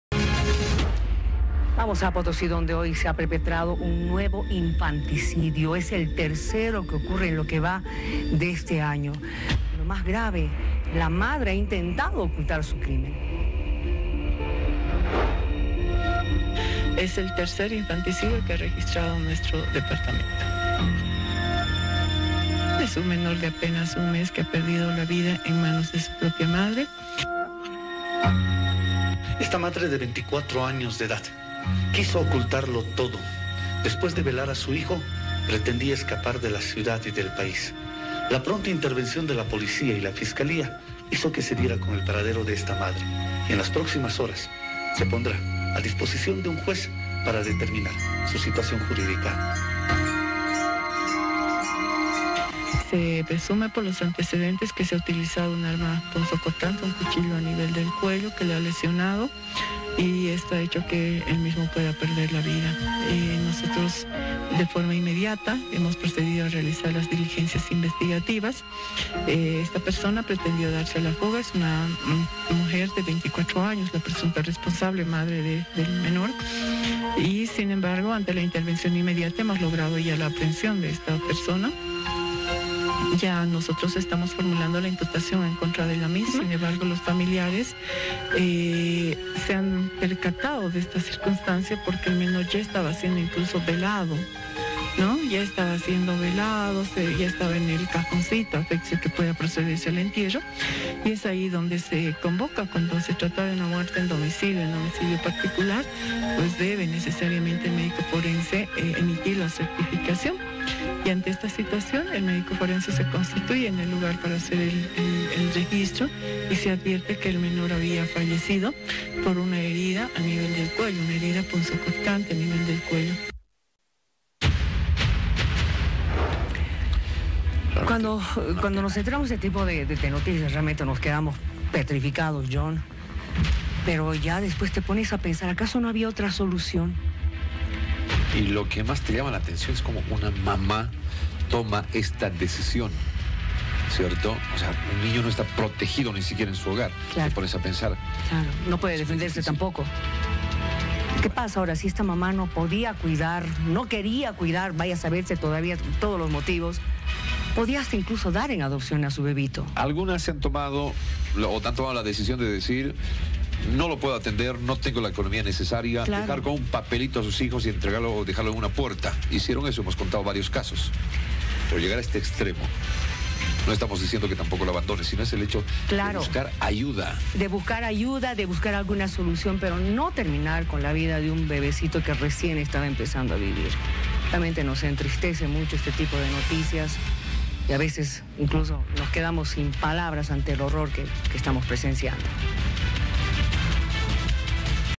Mujer de 24 años asesinó a su bebé de 1 mes
Se registró el tercer infanticidio en Potosí, una madre le quitó la vida a su bebé e intentó ocultar el crimen.